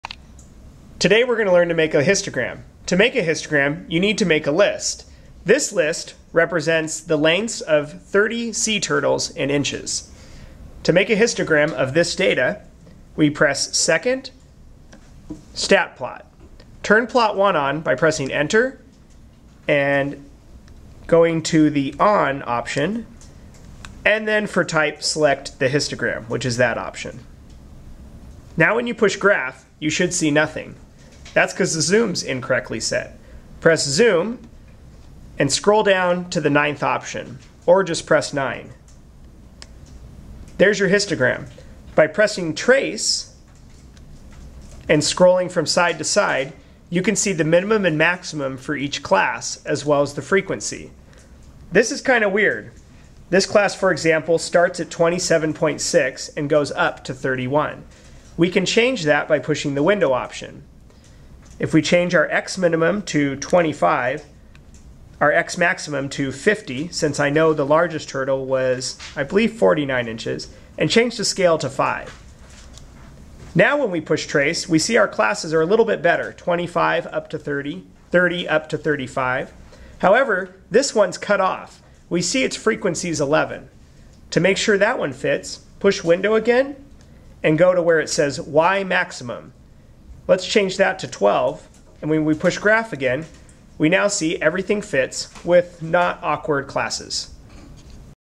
Making Histograms with a TI-84 Plus & Manually Adjusting Classes
This video demonstration shows how to make a histogram with a TI-84, TI-84 Plus, TI-83, or TI-83 Plus. This video also show how to manually adjust the classes the match a histogram made my hand.